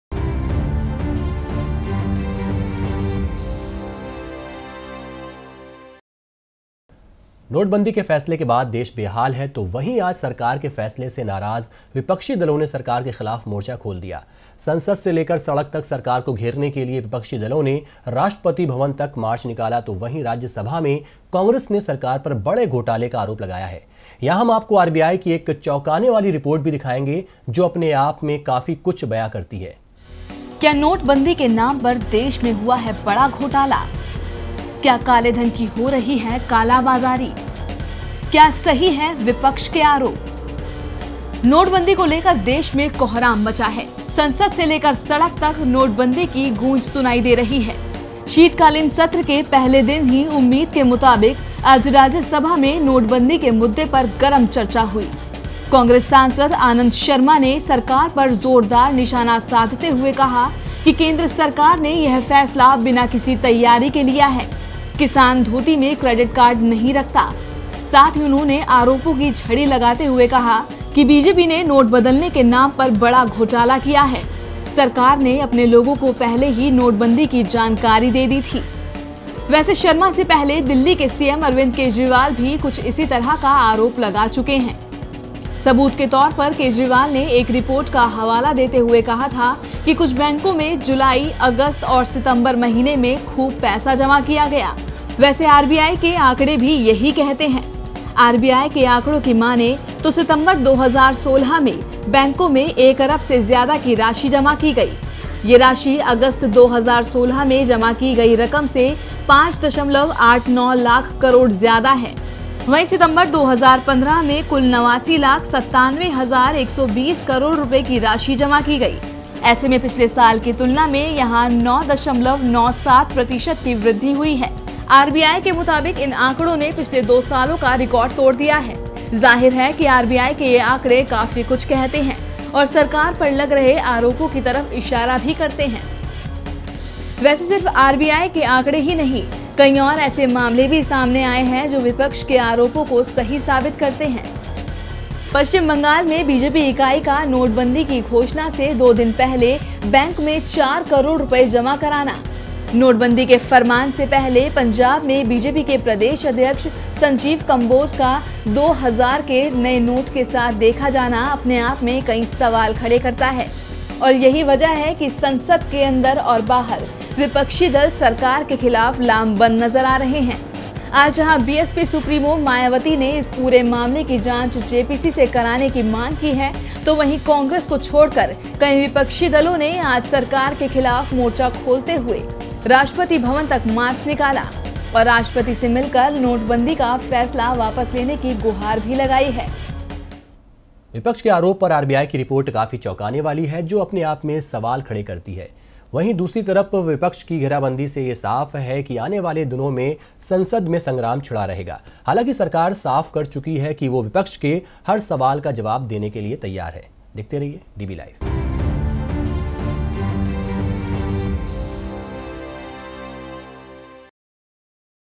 DBLIVE | 16 NOVEMBER 2016 | Currency Crisis to dominate Winter Session of parliament
नोटबंदी के फैसले के बाद देश बेहाल है...तो वहीं इस फैसले से नाराज़ विपक्षी दलों ने सरकार के खिलाफ मोर्चा खोल दिया है...संसद से लेकर सड़क तक सरकार को घेरने के लिए विपक्षी दलों ने राष्ट्रपति भवन तक मार्च निकाला, तो वहीं राज्यसभा में कांग्रेस ने सरकार पर बड़े घोटाले का आरोप लगाया है...यहां हम आपको आरबीआई की एक चौंकाने वाली रिपोर्ट भी दिखाएंगे...जो अपने आप में काफी कुछ बयान करती है...क्या नोटबंदी के नाम पर देश में हुआ है बड़ा घोटाला...क्या कालेधन की हो रही है कालाबाजारी...क्या सही है विपक्ष के आरोप...नोटबंदी को लेकर देश में कोहराम मचा है...संसद से लेकर सड़क तक नोटबंदी की गूंज सुनाई दे रही है,,,शीतकालीन सत्र के पहले दिन ही उम्मीद के मुताबिक आज राज्यसभा में नोटबंदी के मुद्दे पर गरम चर्चा हुई...कांग्रेस सांसद आनंद शर्मा ने सरकार पर ज़ोरदार निशाना साधते हुए कहा कि केंद्र सरकार ने यह फैसला बिना किसी तैयारी के लिया है...किसान धोती में क्रेडिट कार्ड नहीं रखता...साथ ही उन्होंने आरोपों की झड़ी लगाते हुए कहा कि बीजेपी ने नोट बदलने के नाम पर बड़ा घोटाला किया है...सरकार ने अपने लोगों को पहले ही नोटबंदी की जानकारी दे दी थी..वैसे शर्मा से पहले दिल्ली के सीएम अरविंद केजरीवाल भी कुछ इसी तरह का आरोप लगा चुके हैं...सबूत के तौर पर केजरीवाल ने एक रिपोर्ट का हवाला देते हुए कहा था कि कुछ बैंकों में जुलाई, अगस्त और सितंबर महीने में खूब पैसा जमा किया गया..वैसे आरबीआई के आंकड़े भी यहीं कहते हैं...